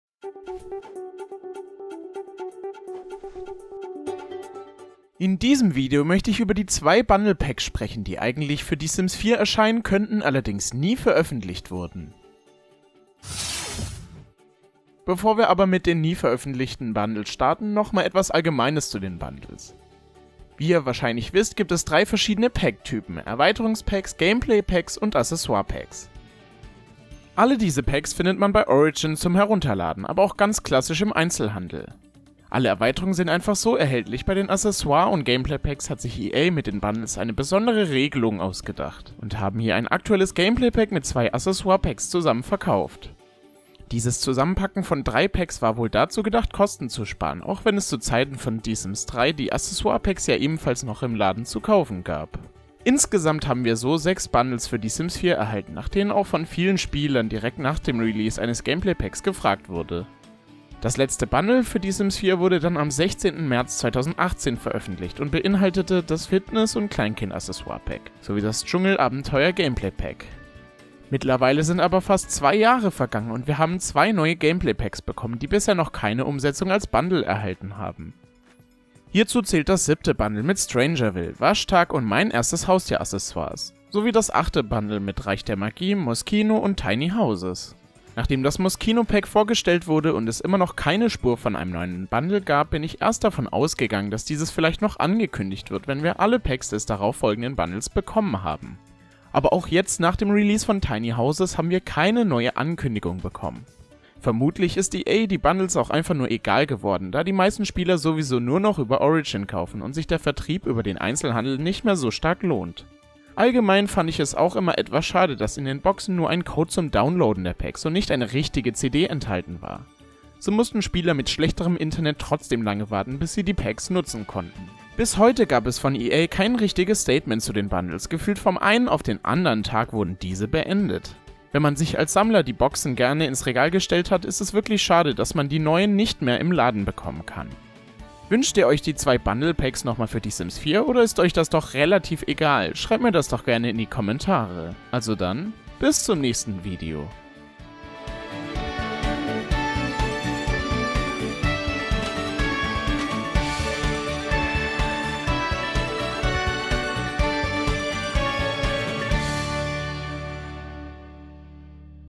Die zwei VERGESSENEN Sims 4 Bundle-Packs! | sims-blog.de
In diesem Video soll es um die zwei verschollenen Bundle-Packs für Die Sims 4 gehen!